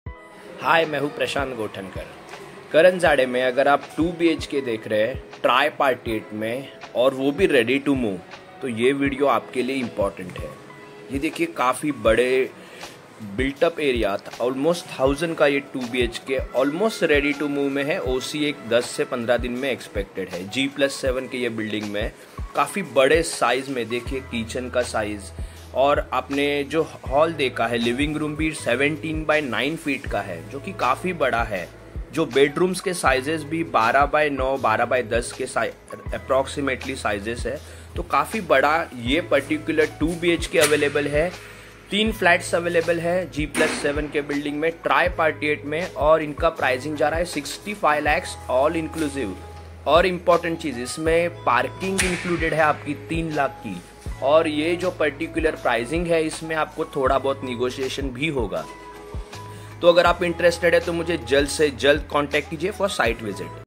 करंजाड़े Tri-party "Ready" 2 BHK @ Lowest price//G+7 New Bldg//Big size 2 bhk//
Karanjade Ready 2 BHK in CIDCO Tri party agreement
Built up area of 960 sq ft
G+7 new building - New construction
OC expected in 15 days
Almost ready to move 2 bhk in karanjade

Ready 2 BHK at lowest price in Karanjade
Price offered includes Parking of 3 Lacs

3 Flats left so you have option of front facing 2 bhk
and back facing also.

For site visit
Prashant Gothankar